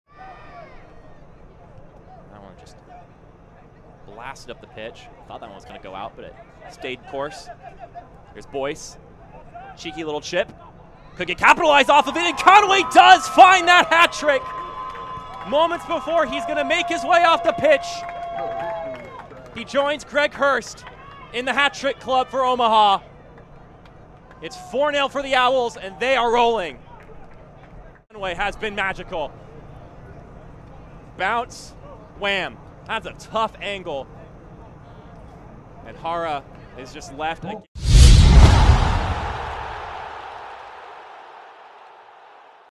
Evan Conway with a Goal vs. South Georgia Tormenta FC
Evan Conway (Union Omaha) with a Goal vs. South Georgia Tormenta FC, 10/01/2021